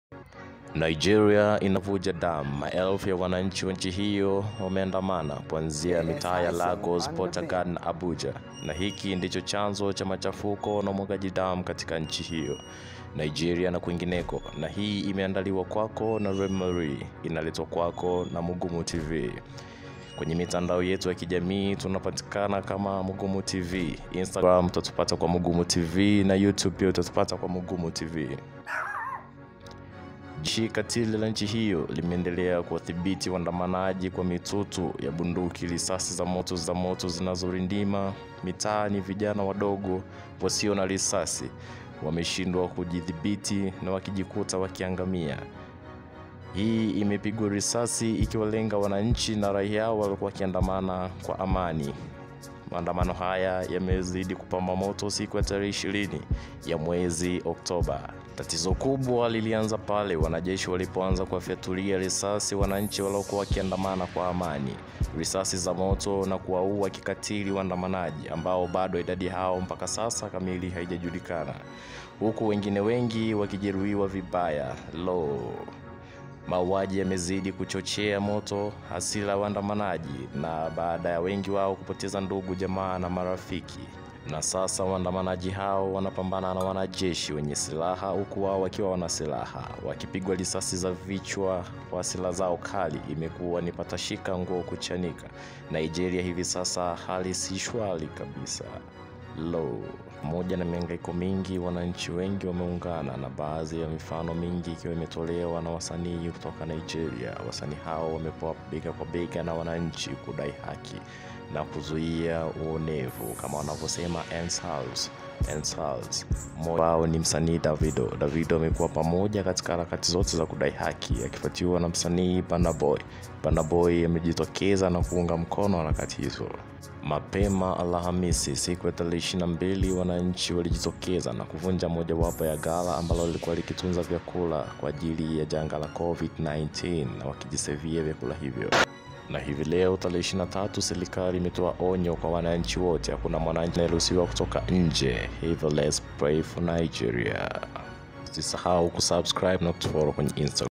HALISISHWALI TAZAMA NIGERIA WATU WANAKUFA KIKATILI MPAKA HURUMA UNAWEZA UKALIA
Tarehe 20 hii ndio siku iliyoleta tofauti kubwa Sana kwa raia wa Nigeria walikuwa wakiandamana kwa amani Ila sasa Hali so sawa watu wamekufa Kama wanyama nasikitisha Sana tuwaombee Nigeria imeandaliwa na @renmar_reee